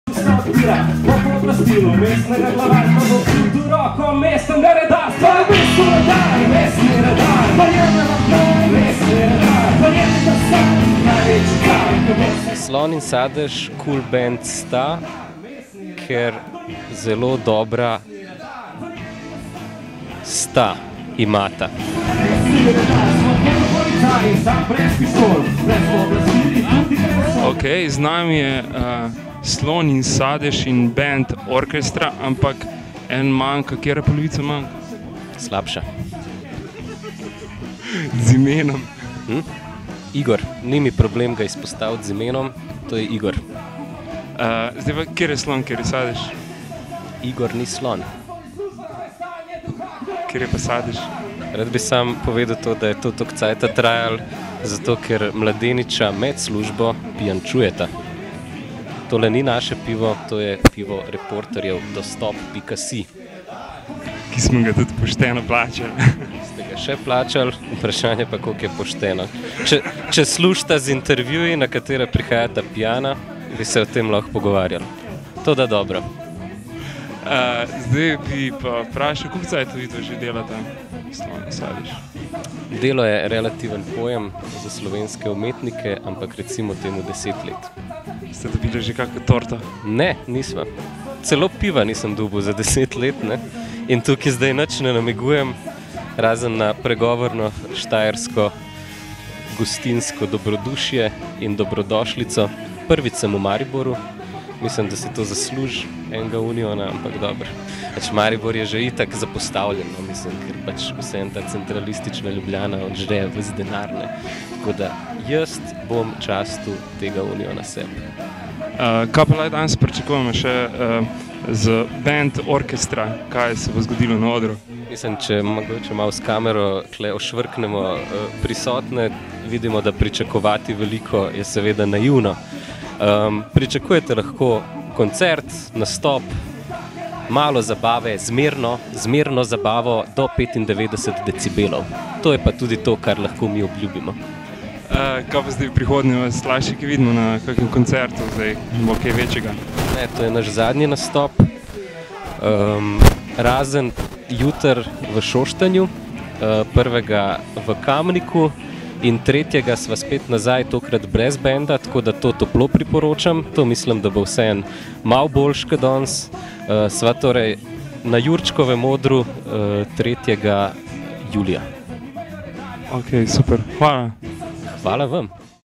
Slon in Sadež @ Lent 2011
S petkom se je pričel Festival Lent. Prvi večer najdaljšega festivala ob Dravi smo posvetili Slonu in Sadežu s svojim rokenrol orkestrom in skupini Ana Pupedan.